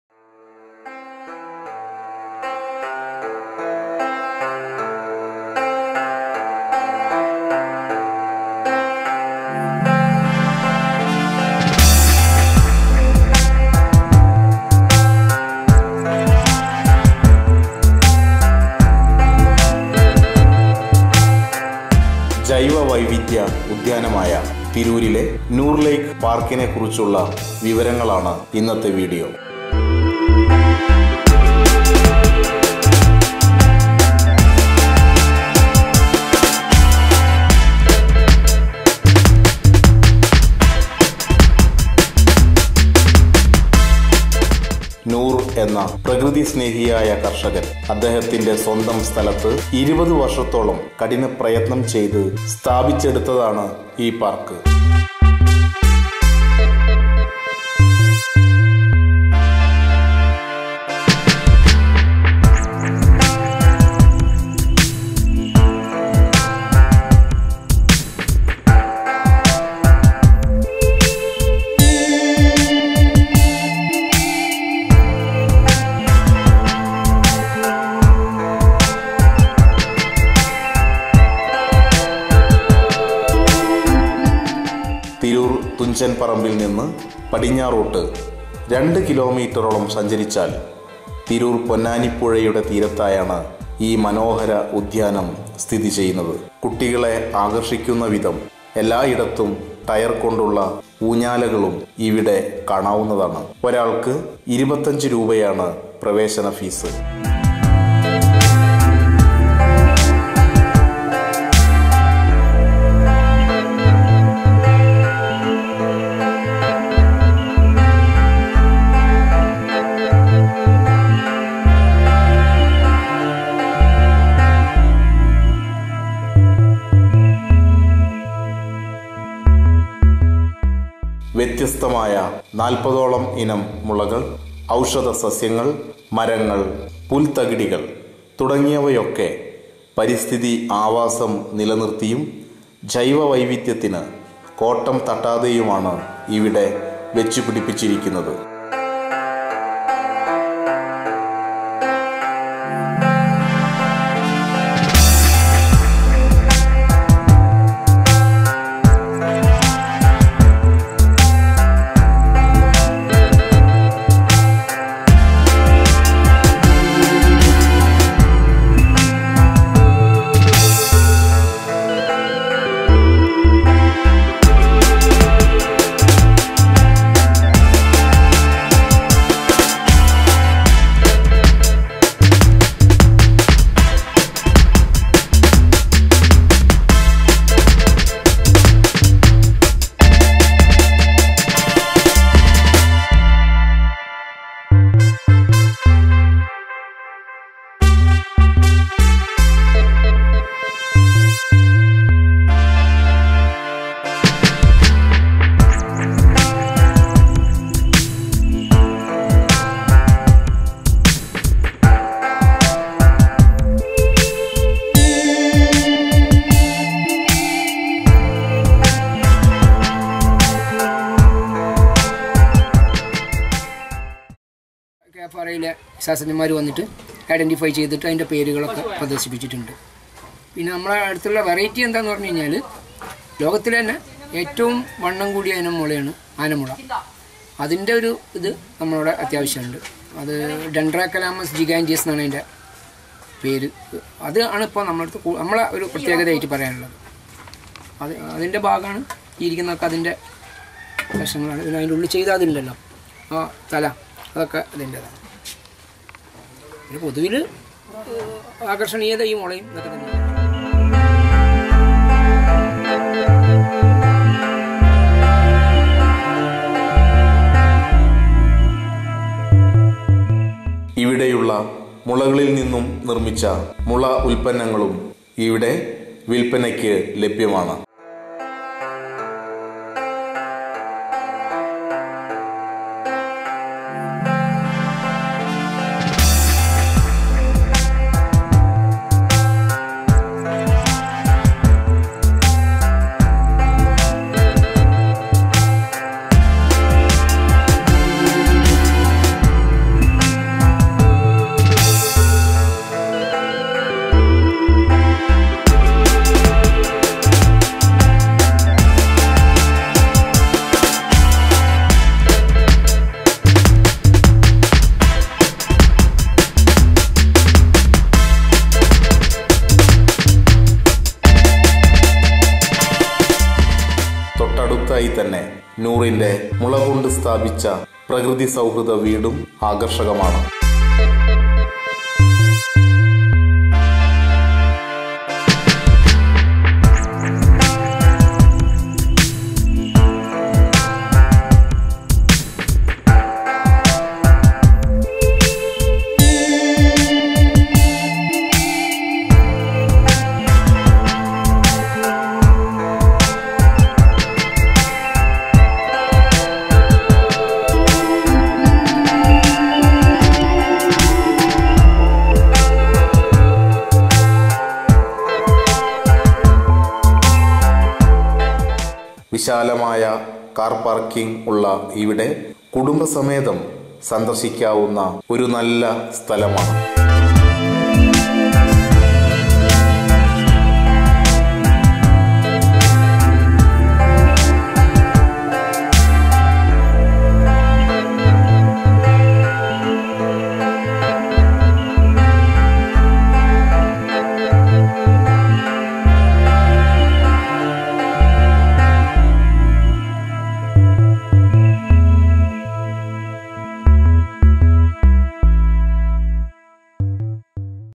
തിരൂർ പച്ചാട്ടിരിയിലെ ജൈവ വൈവിധ്യ ഉദ്യാനം -നൂർ ലെയ്ക്ക്
Noor Lake-
   നാൽപ്പതോളം  ഇനം  അപൂർവ്വ  മുള കളുടെ   വിസ്മയ ശേഖരമാണ്   നൂർ  എന്ന  പ്രകൃതി  സ്നേഹിയായ  കർഷകൻ  സ്വന്തം  സ്ഥലത്ത്  വെച്ച്  പിടിപ്പിച്ചിരിക്കുന്നത്.

  കാഴ്ച്ചക്കാർക്ക്   കൗതുകം  പകരുന്നതാണ് . പ്രകൃതിയുടെ  താളത്തിനൊത്ത്  മരങ്ങളും  മുളകളും  ഔഷധ സസ്യങ്ങളും  എല്ലാം  ചേർന്ന  ഒരു  ജൈവ  വൈവിധ്യ  ഉദ്യാനം .

കുടുംബ സമേതം തീർച്ചയായും വന്നിരിക്കാവുന്ന,. സന്ദർശിക്കാവുന്ന ഒരു നല്ല പാർക്കാണ് …….